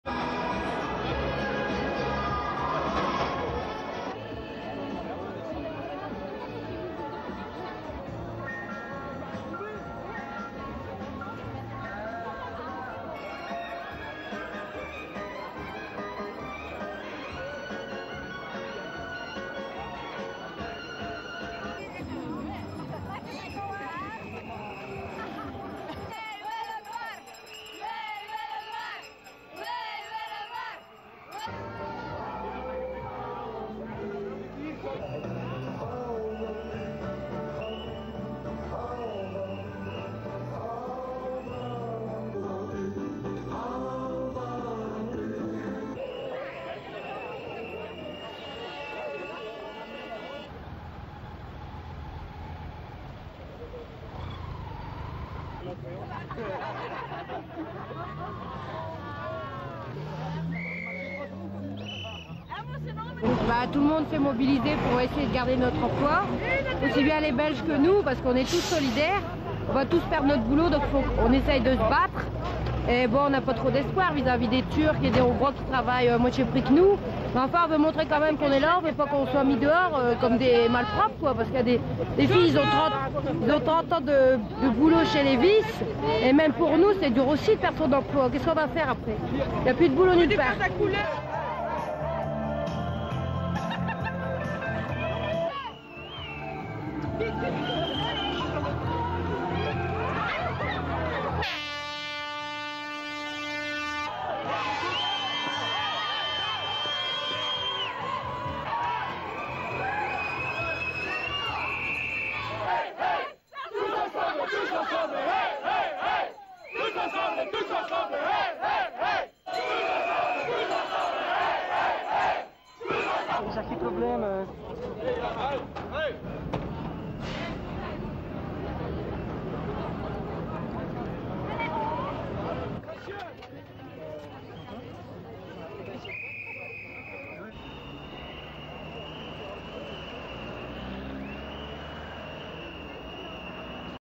BELGIUM: BRUSSELS: WORKERS PROTEST OUTSIDE LEVI STRAUSS HQ
(5 Oct 1998) French/Nat

More than two thousand protesters in Brussels demonstrated outside the European headquarters of Levi Strauss on Monday.

They were demanding that the blue jeans company overturn its decision to close four plants in Belgium and France.

More than one and a half thousand people will lose their jobs if the plants are closed down.

Angry workers protested outside a building in Brussels on Monday where the board of Levi Strauss was holding a meeting.

Levi Strauss announced last week it wants to cut 1,560 jobs in three Belgian and one north French factory as part of a European restructuring plan.

Unions have demanded management save at least part of the jobs.

The workers have been on strike since the company announced its plans last week.

After demonstrating outside the board meeting, they took buses to the company's European headquarters to continue their protest.

Company officials said production facilities in Belgium and France were singled out because of high labour costs and overcapacity.

But the workers are furious and very worried about their futures.

SOUNDBITE: (French)
"Everybody is here and have mobilised to keep our jobs, Belgium people as well as the French, because we are all in this together, we are all going to lose our jobs so we are trying to fight but we don't have much hope up against Turkish and Hungarian people who are working for half the wages that we earn. But we just want to show that we are here, we don't want to be pushed around and treated like rubbish. Some women have been working for Levis for 30 years and even for us it is hard. Nobody can get jobs these days. What are we going to do afterwards? There's no work anywhere. "
SUPER CAPTION: Protestor

The demonstrators made their way to the entrance of the headquarters and began pelting it with eggs.

While some protestors got inside and continued their noisy demonstration, others symbolically set fire to Levi's jeans' in front of the headquarters.

About 7,500 of the company's 30-thousand employees are based in Europe.

The layoffs will represent about 20 percent of the company's European staff.